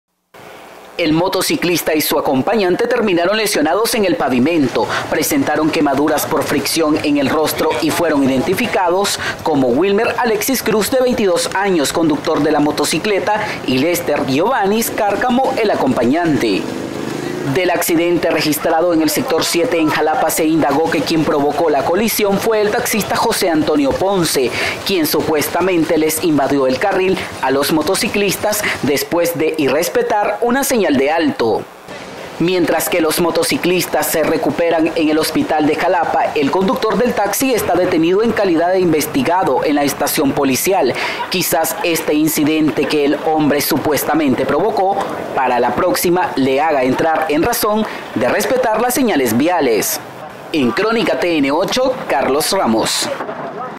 Nicaragua: Motociclistas se accidentan en Jalapa por taxista que irrespetó un ALTO
Un accidente entre dos vehículos dejó como saldo a una pareja de motociclistas lesionados en el sector 7 del municipio de Jalapa, en Nueva Segovia.

Noticiero Crónica emitido el 24 de marzo 2020